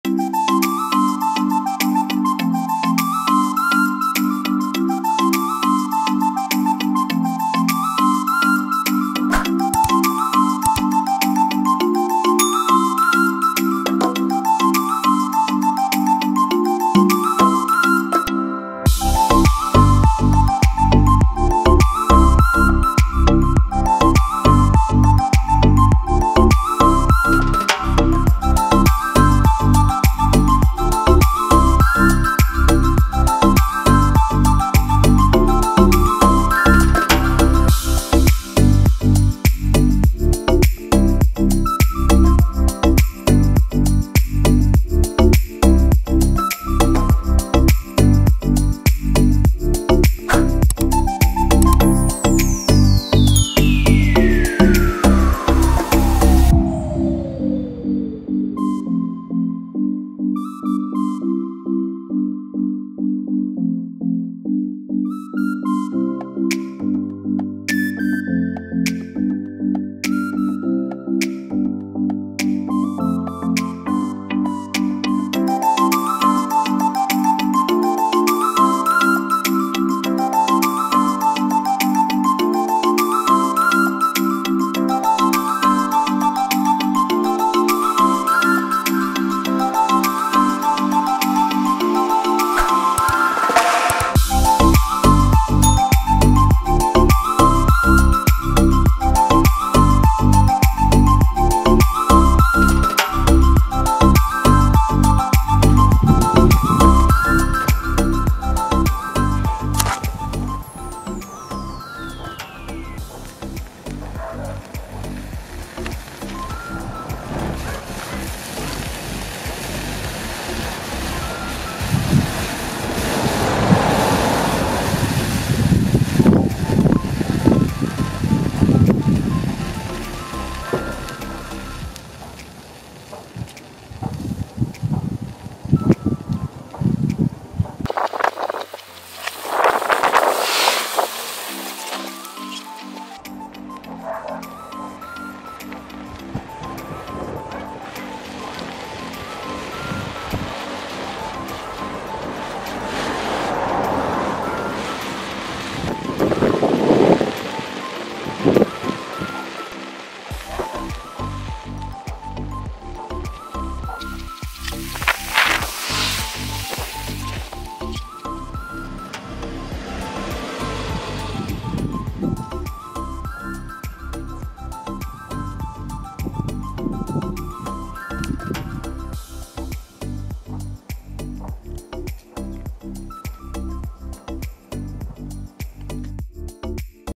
APPLE Match Powder Chain Reaction Amazing Fire Art 성냥 가루로 사과 만들기
warning : Never in the house.
Children do not follow.

[Music]

The image was made with a 4K camera.
The video is copyrighted by 'fireshow'.
It is possible to share this image, but it is not possible to change it without permission.